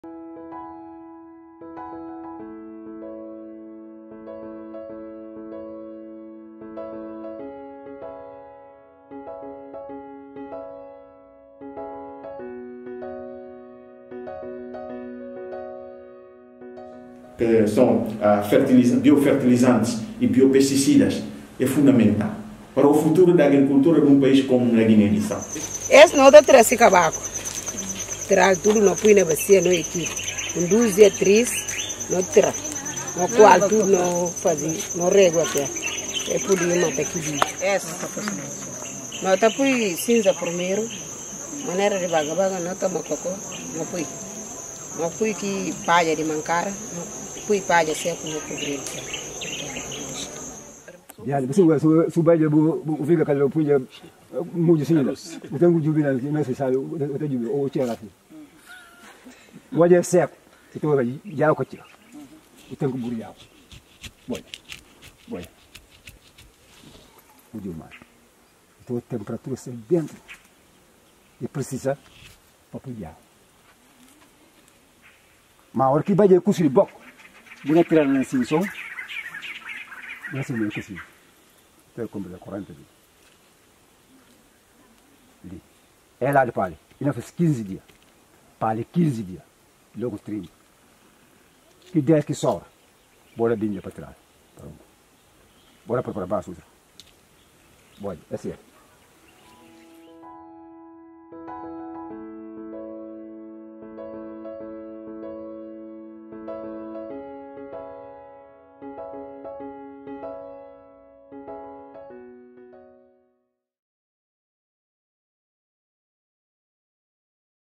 Альтернативы опасным пестицидам в Гвинее-Бисау
This video outlines some examples of how farmers in Guinea Bissau are successfully using organic alternatives to chemical pesticides and fertilizers.